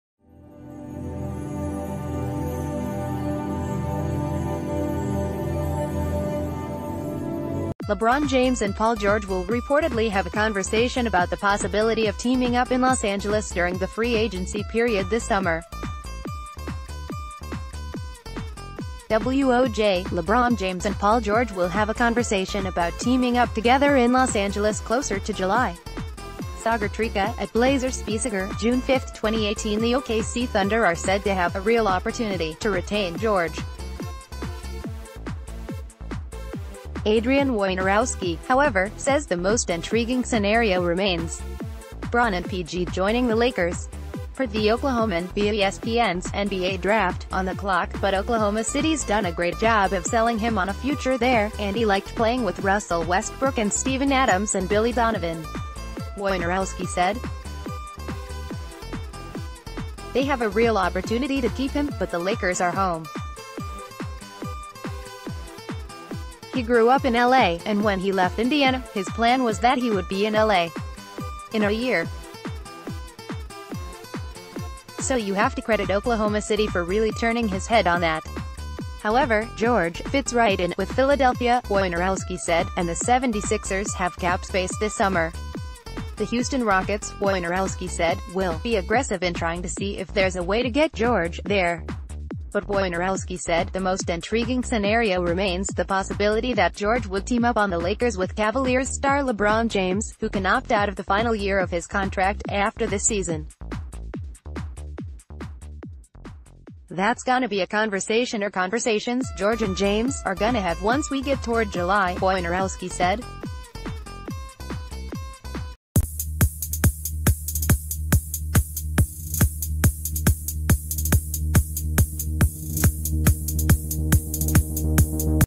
Breaking News | Report: LeBron James and Paul George to Discuss Teaming Up
LeBron James and Paul George will reportedly have a conversation about the possibility of teaming up in Los Angeles during the free agency period this summer. Woj: LeBron James and Paul George will have a conversation about teaming up together in Los Angeles closer to July. — Sagar Trika (@BlazersBySagar) June 5, 2018 The OKC Thunder are said to have “a real opportunity” to retain George. Adrian Wojnarowski, however, says “the most intriguing scenario remains” Bron and PG joining the Lakers. Per...